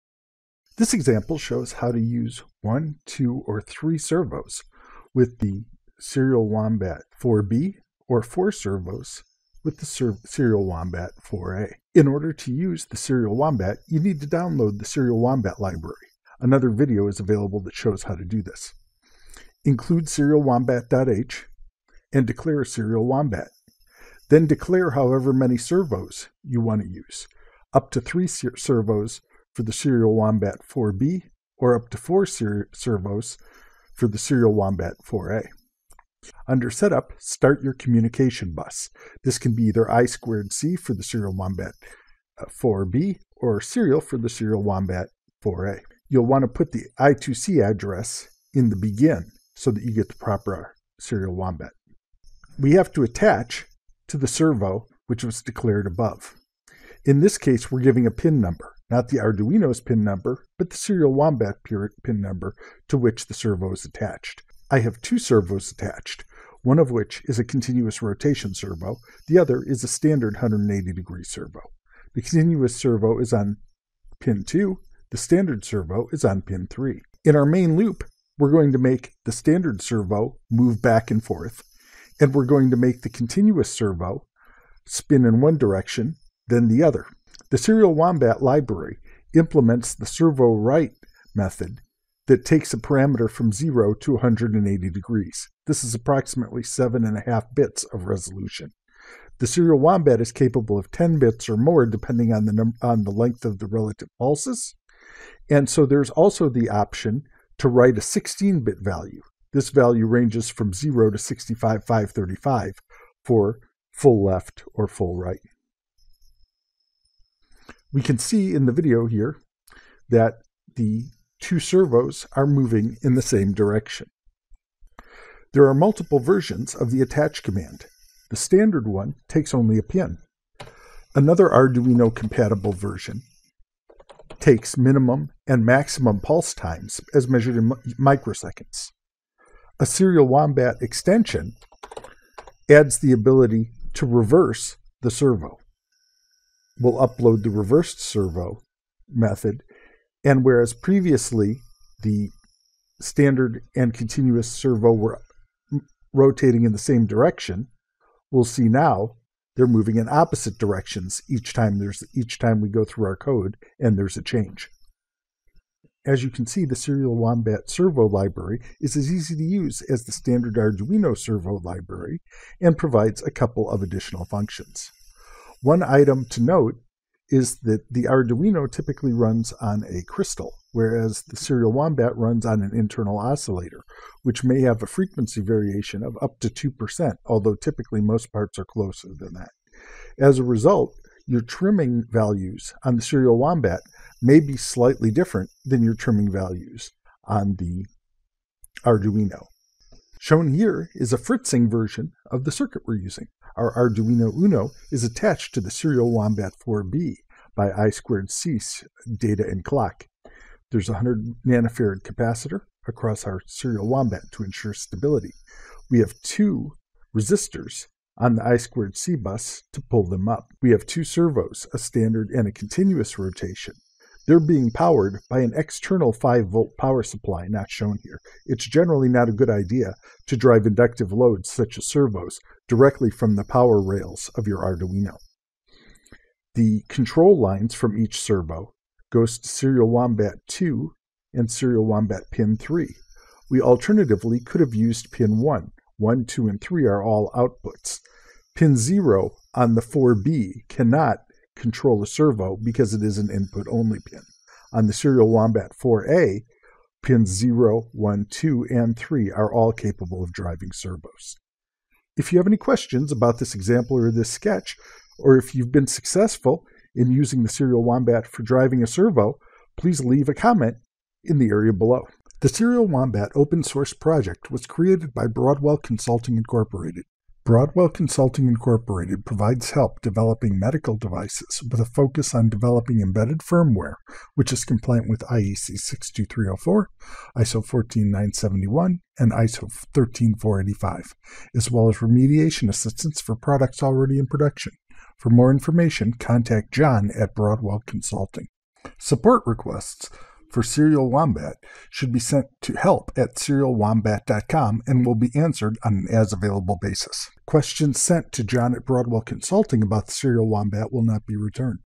Adding I2C based Servo Control Lines to your Arduino Project with the Serial Wombat 4B Chip
This video goes with the Serial Wombat 4B Servo example sketch in the Serial Wombat Arduino Library.  It shows how to use a Serial Wombat chip to add additional Servo Control lines to your Arduino Project.  The Serial Wombat chip is capable of pulse precision which is significantly finer than the base Arduino Library.

The Serial Wombat Firmware C file that implements this pin mode is available for reference here:

The Serial Wombat chip is a Smart I/O Expansion chip for Arduino, Raspberry Pi, CircuitPython, native embedded systems, PC, or any other system that can communicate by UART or I2C.

0:00 Intro
0:19 Arduino Sketch Review
2:06 Variations on the Attach Command
2:20 Reversing the Servo
3:19 Circuit Diagram Review
4:32 Broadwell Consulting Inc